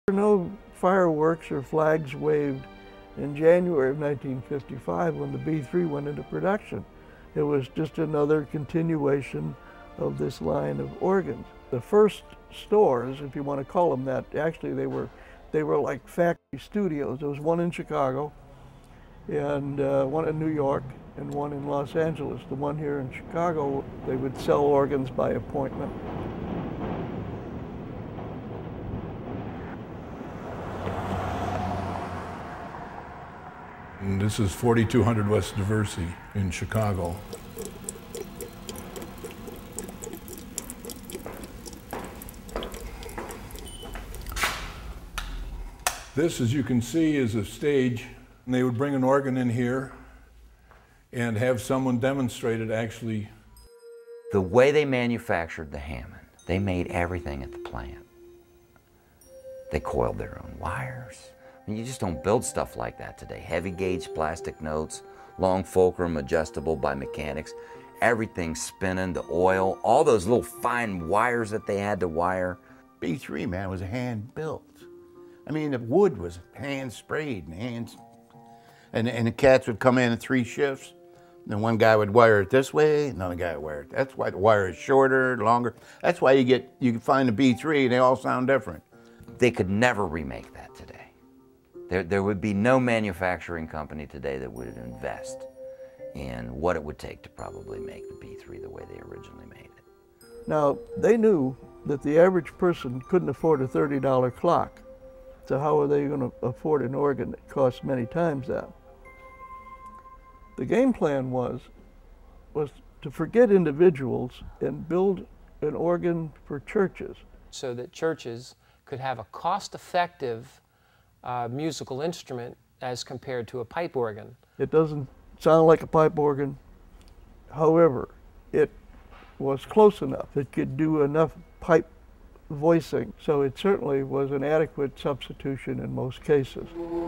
Killer B3 - Hammond Organ Plant Chicago
This clip features Chicago Hammond Organ Historian Harvey Olsen talking about the birth of the B3.  Also, get a candid peek inside the old Hammond Plant in Chicago.  "Like" us on Facebook.  Search Killer B3 for more information. Please share with other fans of Hammond Organ music. Thank you for your support.